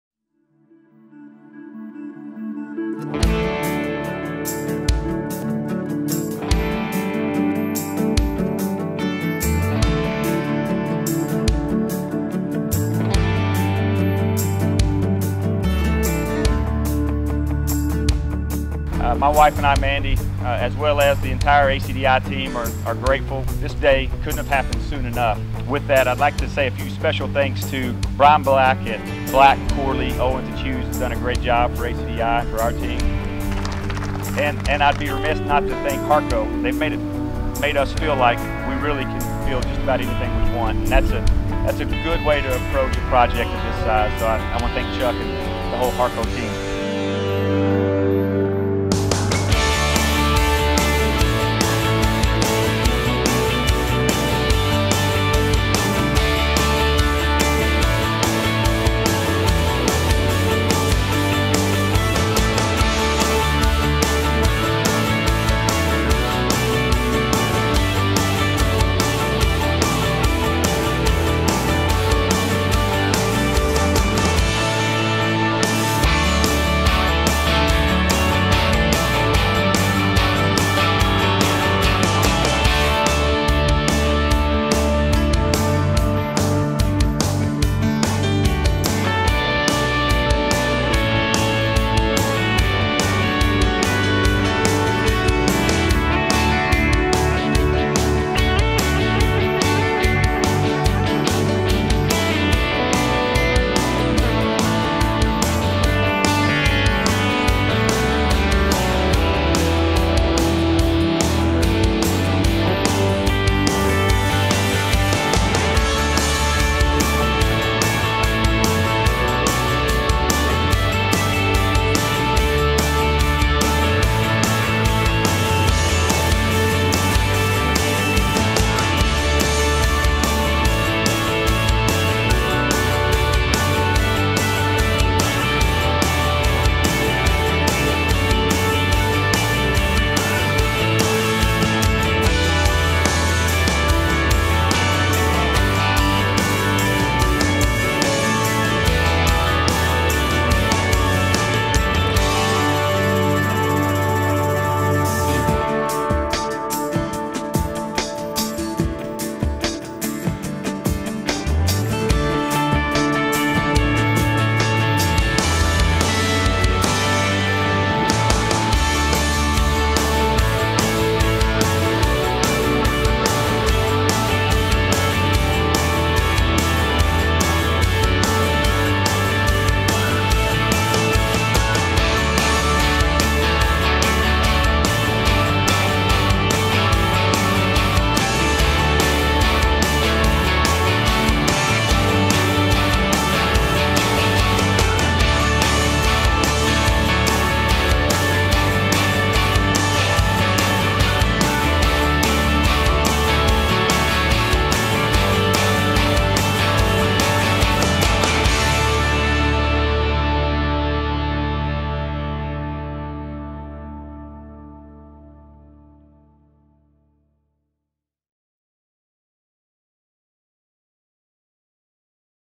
Harco - ACDI Start to Finish
Here is a look at construction of ACDI office headquarters in Benton, Arkansas, from beginning to end. Harco was excited to serve as construction manager for this beautiful new building.